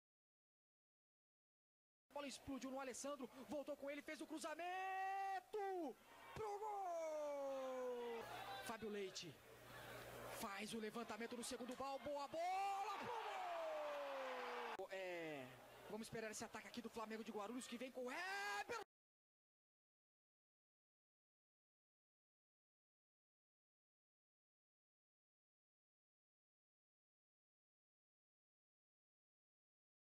CAMPEONATO PAULISTA 2017 SERIE A3 MARÍLIA 1 X 2 FLAMENGO-SP
PAULISTÃO SERIE A3 2017  - GOLS DO JOGO

Estádio Bento de Abreu - Marília - SP

Jogo da 17° Rodada Marília 1 x 2 Flamengo de Guarulhos

TRANSMISSÃO: TV FPF-SP

Dia: 05/04/2017

HRS: 20:00

18° RODADA, 09/04/2017 - SÃO JOSÉ DOS CAMPOS X MARÍLIA

18° RODADA, 08/04/2017 - DESPORTIVO BRASIL X FLAMENGO

O JOGO

FLAMENGO GANHA MAS MESMO COM A VITORIA ESTÁ REBAIXADO PARA A SEGUNDONA DO ANO QUE VEM, MAS COM A DERROTA O MAC FICA SO COM O GOSTINHO AMAGO NA BOCA !!!

* Parceiros *

João Dutra